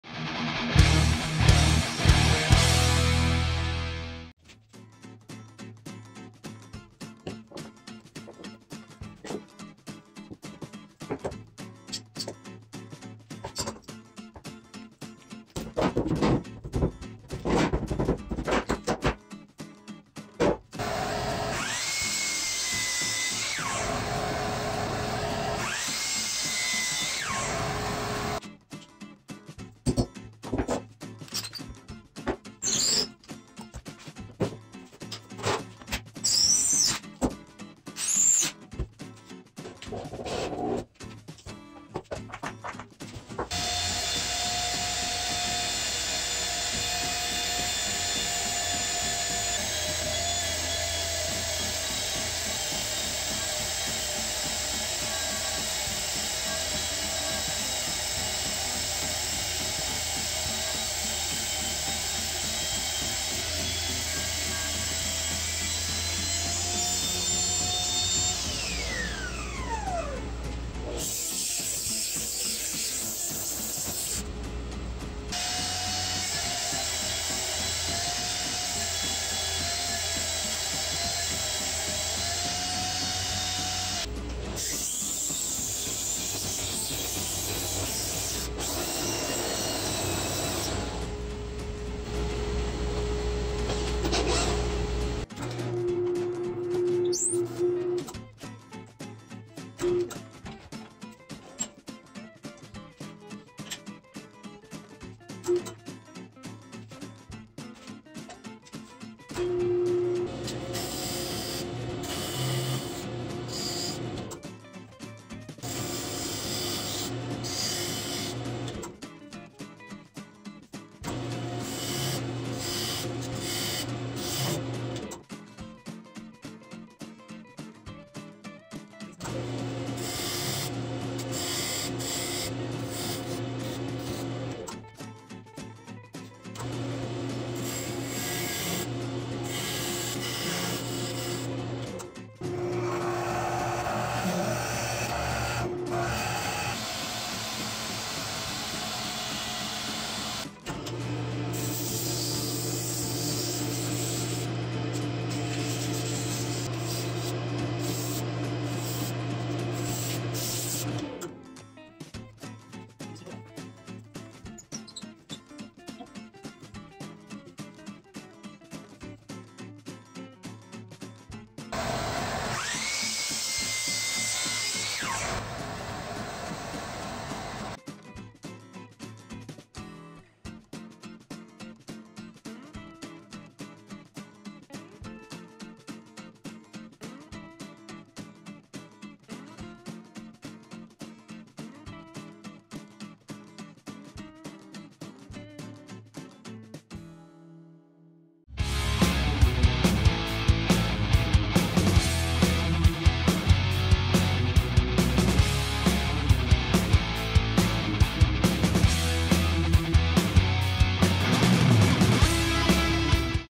How to Make a Wood Vase - Lathe Project - Gift Idea
I glued up 2 pieces of 3/4" oak and maple to make a blank which I could bring to my table saw lathe to rough out a cylinder.  I then mounted the blank on my wood lathe via my homemade center
and added decorative details and sanded to 220 grit. I finished this project with mineral oil.